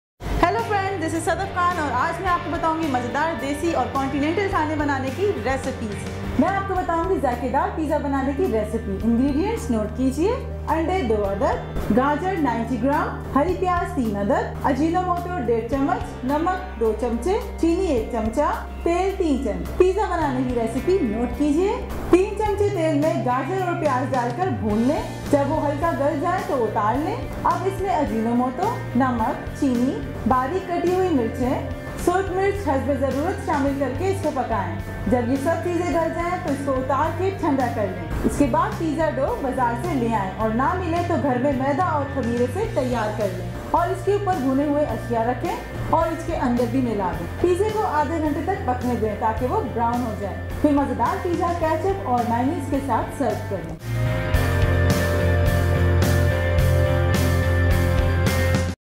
Jhat Pat Pizza Banaye Sirf 5 Mint Mein | Sadaf Khan
Pizza
Desi & Continental Recipes
Urdu Recipe

A quick recipe for all those love to cook for themselves and for the families.

Must Watch Latest Cooking Recipes on “Food court”